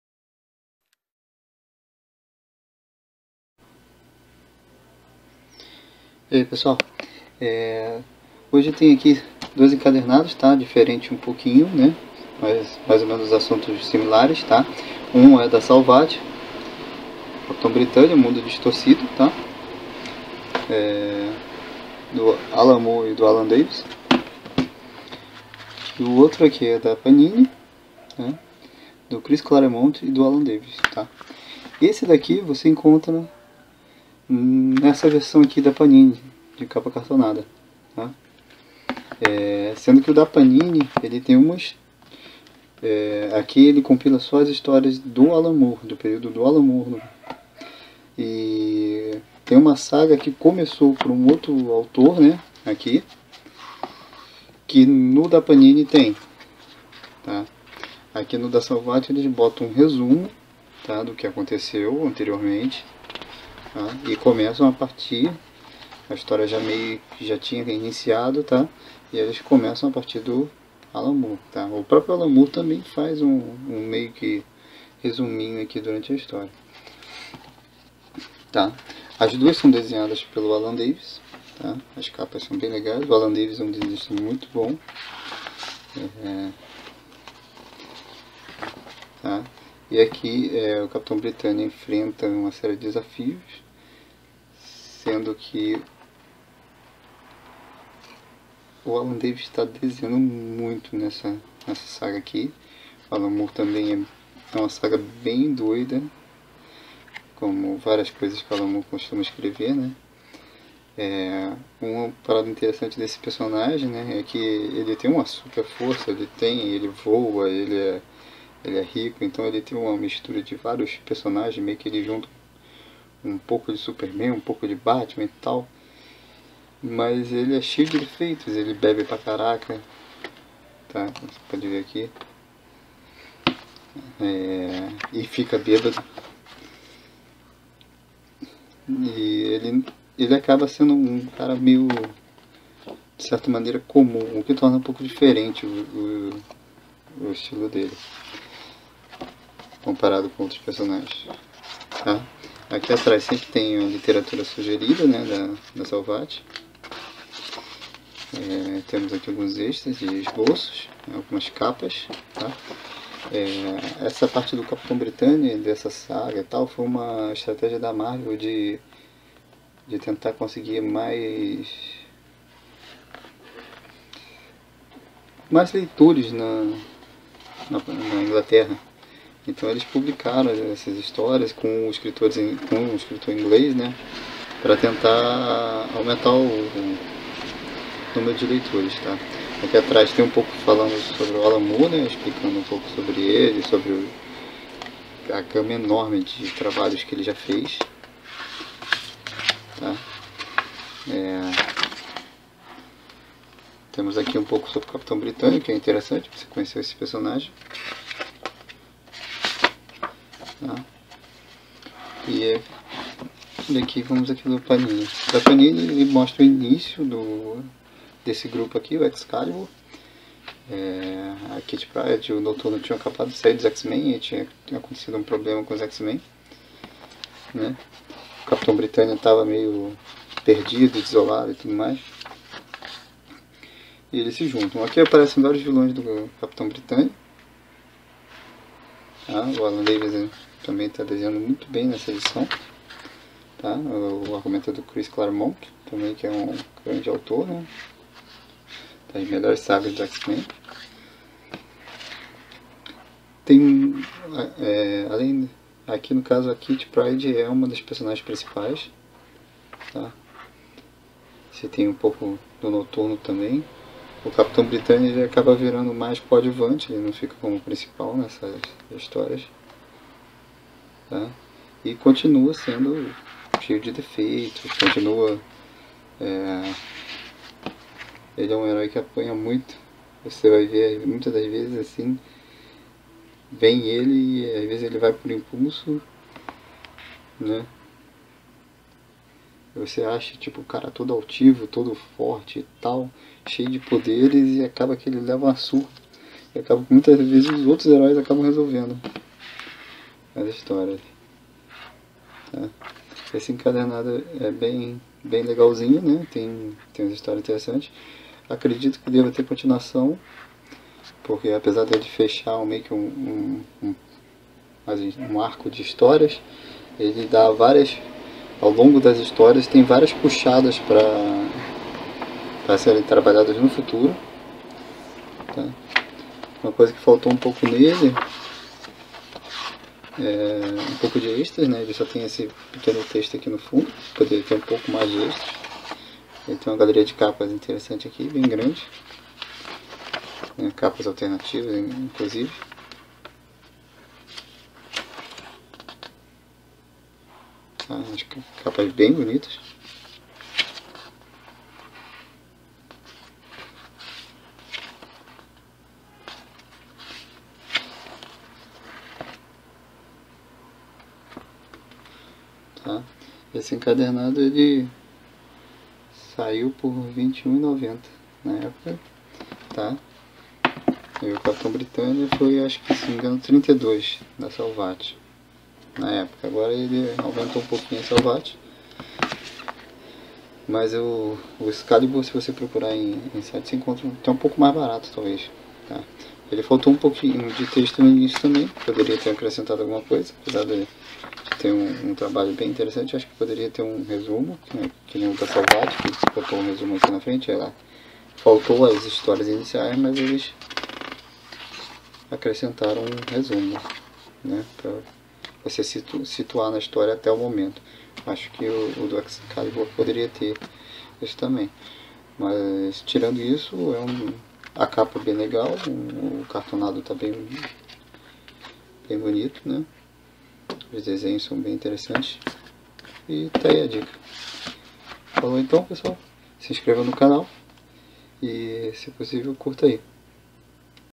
CAPITAO BRITANIA e EXCALIBUR
Breve resenha sobre os encadernados do Capitão Britânia escrito por Alan Moore e desenhado por Alan Davis publicado pela Salvat e do Excalibur  escrito por Chris Claremont e desenhado por Alan Davis publicado pela Panini